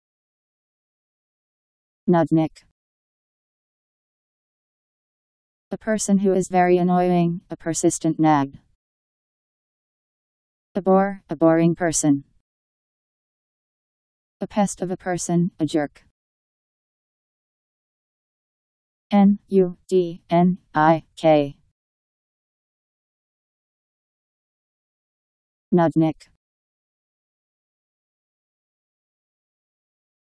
Nudnik Meaning
Video shows what nudnik means. A person who is very annoying; a persistent nag.. A bore; a boring person.. A pest of a person; a jerk..  nudnik pronunciation. How to pronounce, definition by Wiktionary dictionary. nudnik meaning. Powered by MaryTTS